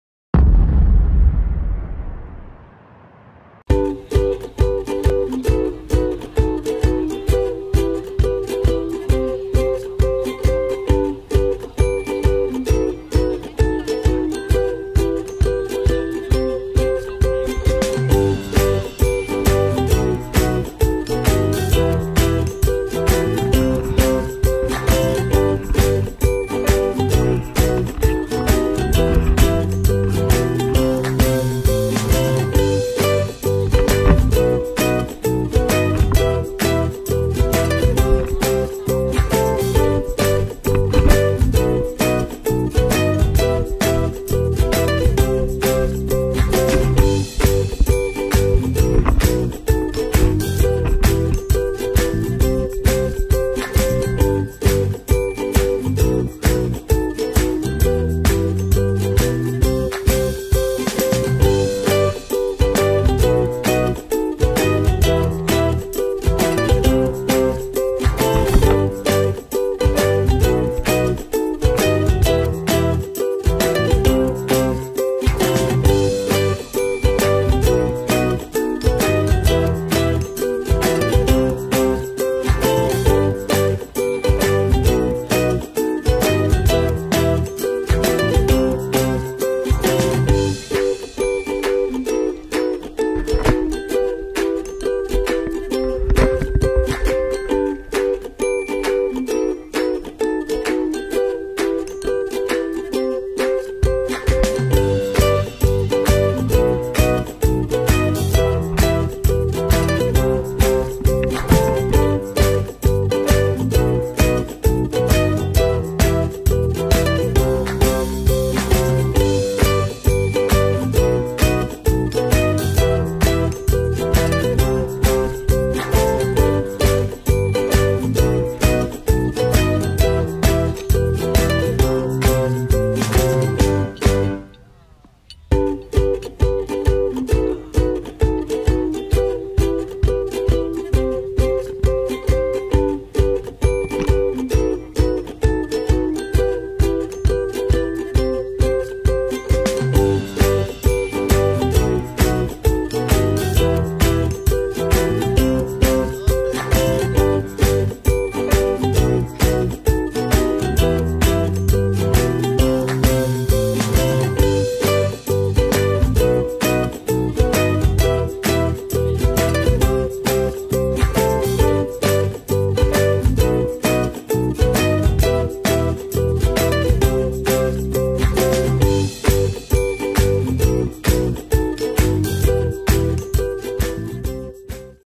GoPro - Score Cupid Dash 2015
Happening and fun run at SCORE Cupid Dash 2015, Kuala Lumpur